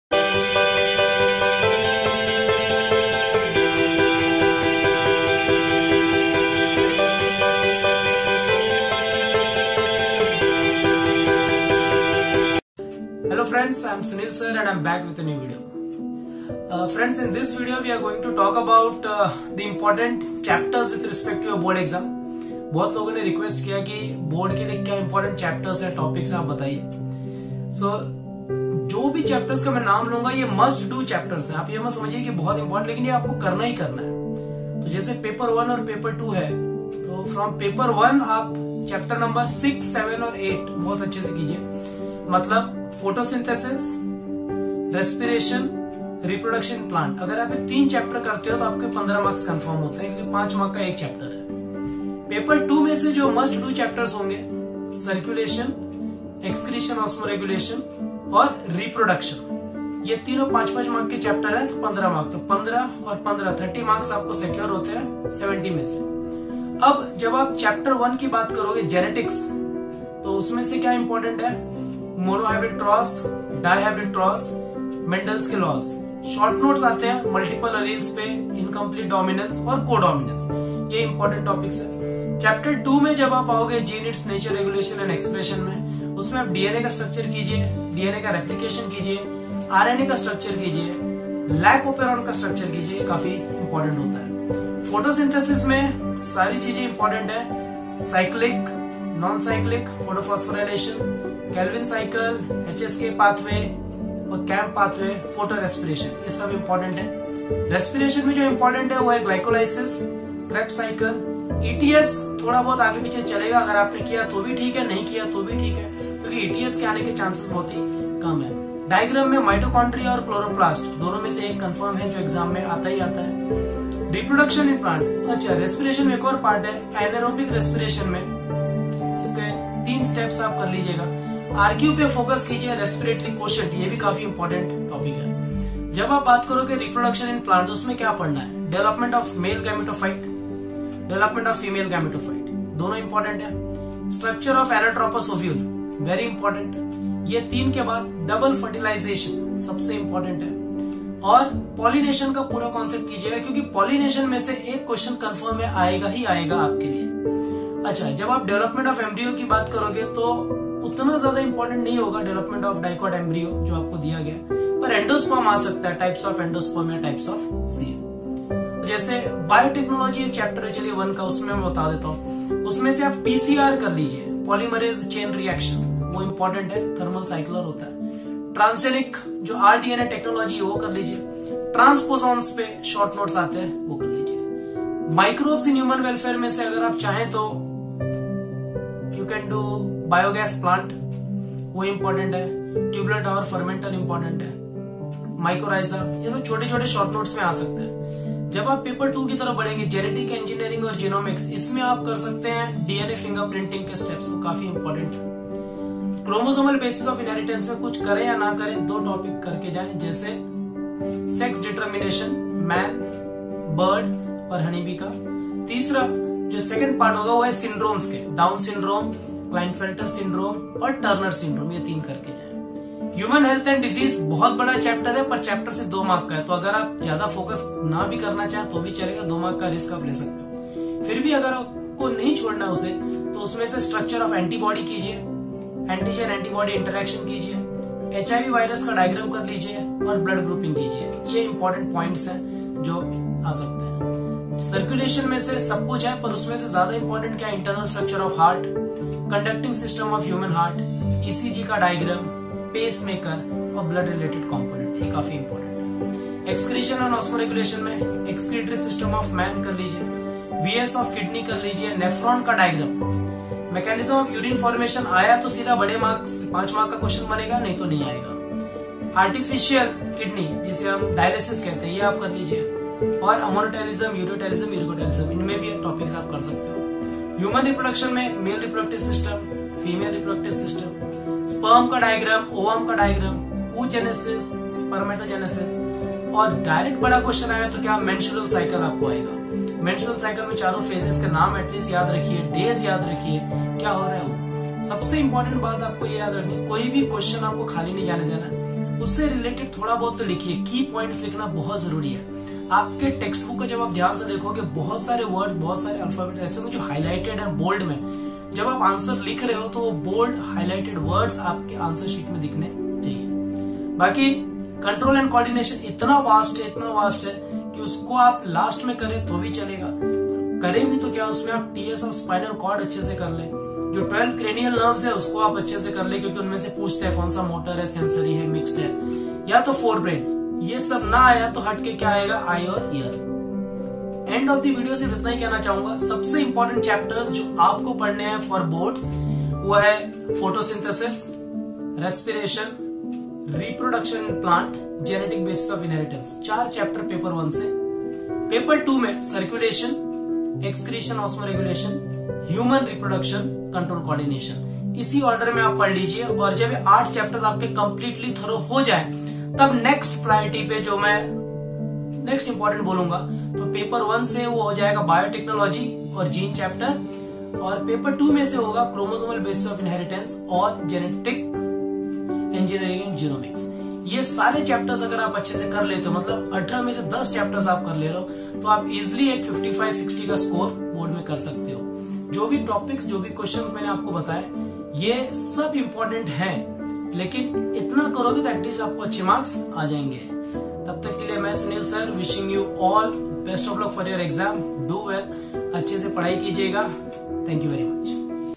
MOST IMPORTANT CHAPTERS FOR BIOLOGY ( HSC BOARD)
This video is based on previous year importance and marking scheme given from board.

The chapters are most probable and topics are must do to score good marks in biology.

Keep studying
Keep Rocking
Faila do with friends and everyone.